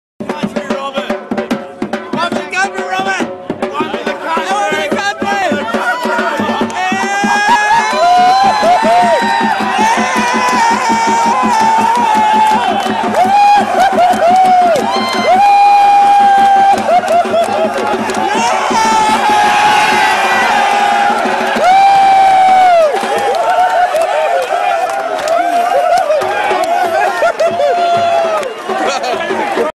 Bob hawke sculling beer SCG - India V Australia.mp4
His academic achievements were complemented by setting a new world speed record for beer drinking: he downed 2 1/2 pints - equivalent to a yard of ale - from a sconce pot in eleven seconds as part of a college penalty. WIKI